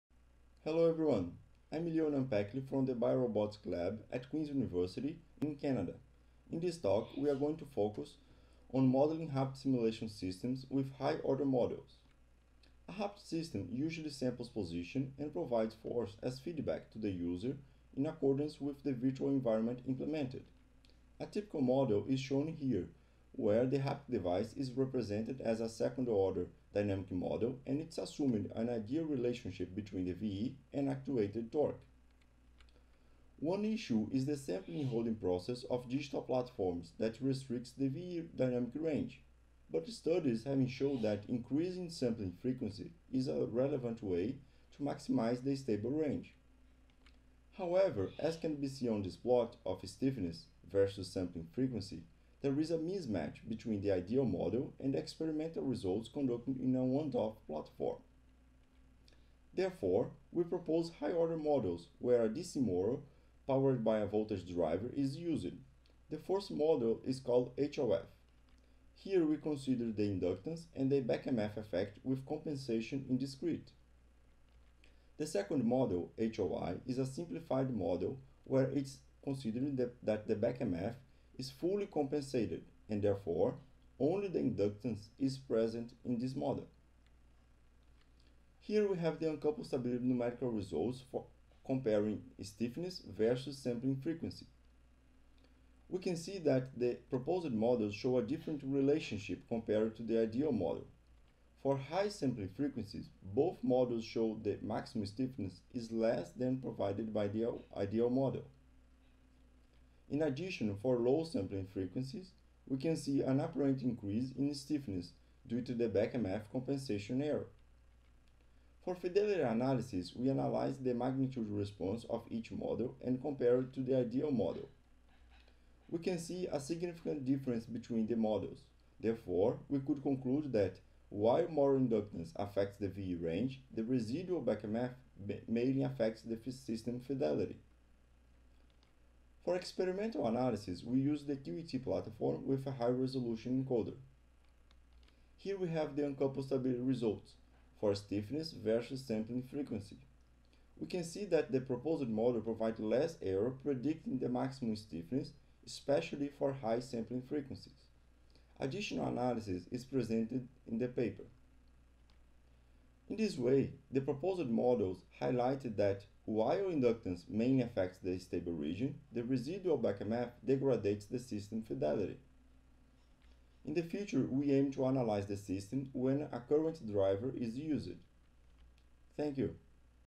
Pecly et al, Effect of Voltage Driver on Kinesthetic Haptic Systems, IEEE Haptics Symposium 2022
L. Pecly, K. Hashtrudi-Zaad, Effect of Voltage Driver on Uncoupled Stability and Fidelity of Kinesthetic Haptic Systems, Proceedings of IEEE Haptics Symposium 2022
Abstract: The stability and fidelity of haptic simulation systems have been studied for a safer and more faithful haptic interaction with virtual environments. The theoretical analysis, which use a second-order model for the actuator does not match well with the experimental results at high sampling rates. In this work, we propose more accurate models and analyze the uncoupled stability and fidelity for a system actuated by a permanent magnet DC motor and its voltage driver when implementing a linear viscoelastic environment. Through theoretical work and experiments, we show that the stability and fidelity of the system are more accurately represented when a high-order dynamic model is used for the motor and its driver.